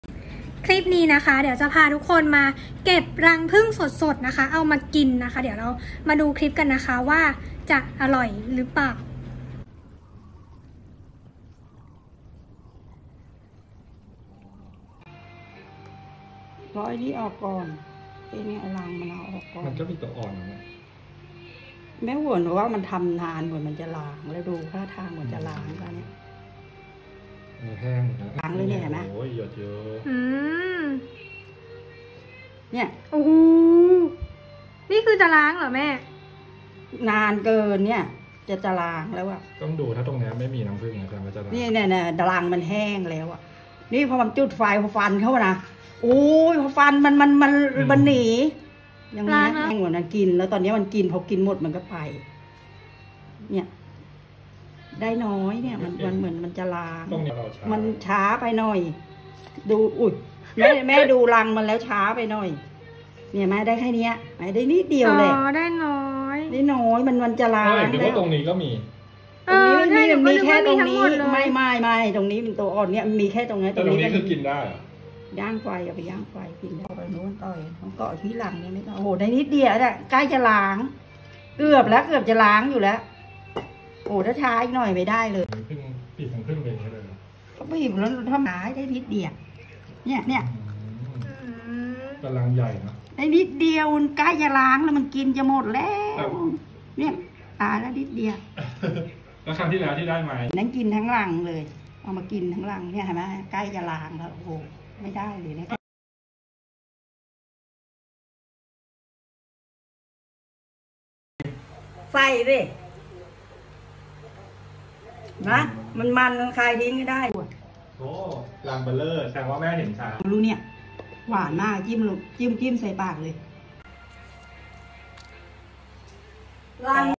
คลิปนี้พามาเก็บรังผึ้งสดๆ เอามาลองชิมกันจ้า ครั้งแรกเลย มาดูคลิปกันน้า
เก็บรังผึ้งครั้งแรก มาดูกันจ้า
แม่ชอบมาก อิอิ